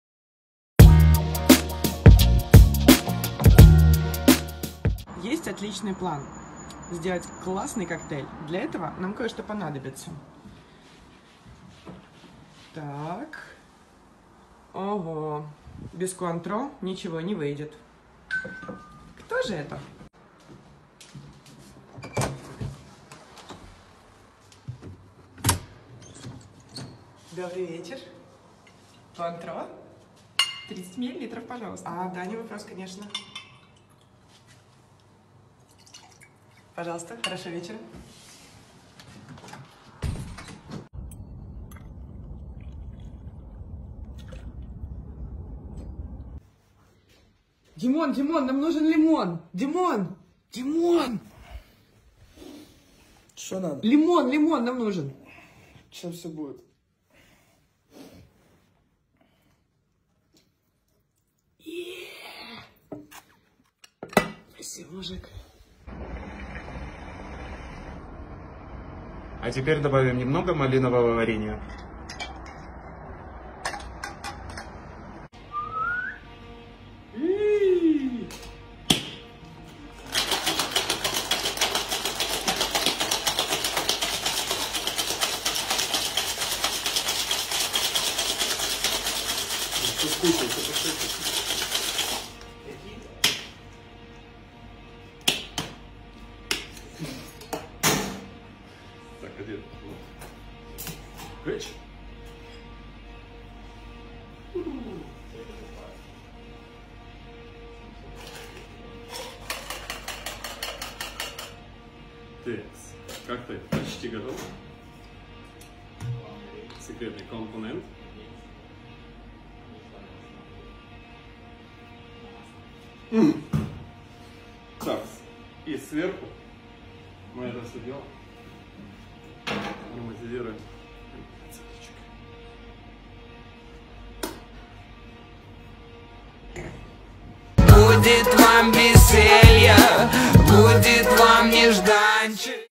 😷🍹 Коктейль "LOCKDOWN COSMO" от бара "Who&Why. Drinkery". Готовим дома в Карантин. Киев.
Бар "Who&Why. Drinkery".
г. Киев, ул. Ярославов Вал, 21а/20.